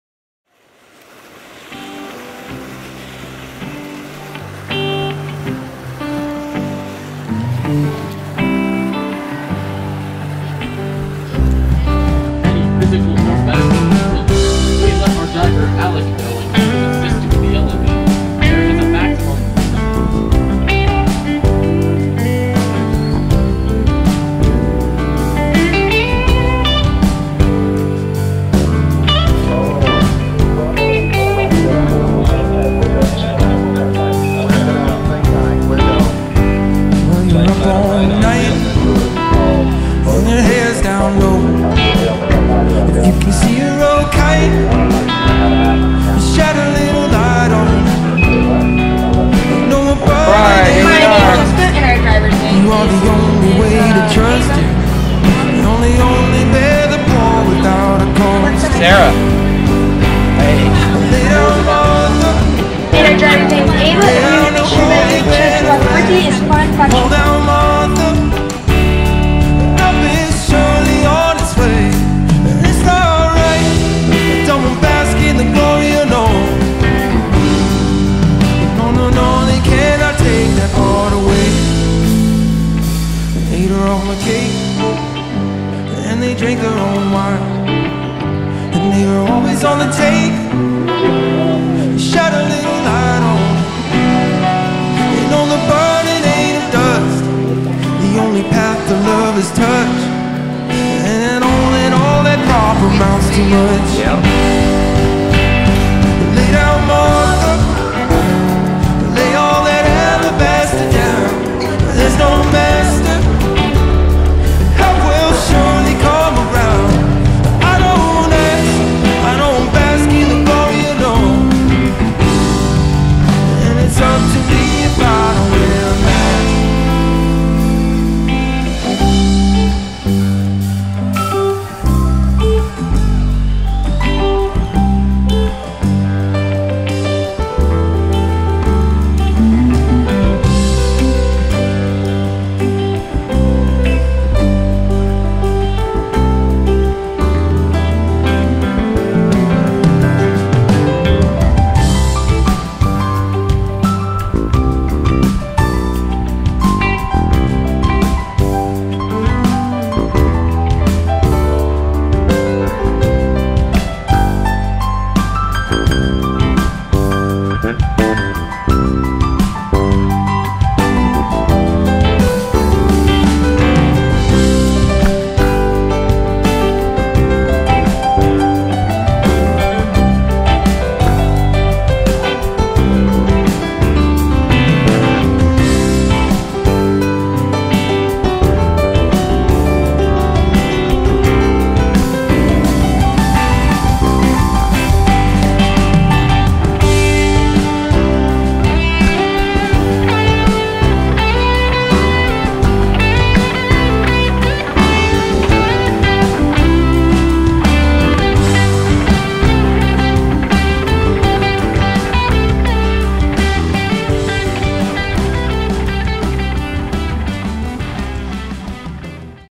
Houston July 2024 - Johnson Space Center and Natural History Museum
A visit to Johnson Space Center and the Natural History Museum. A few clips and a photo slideshow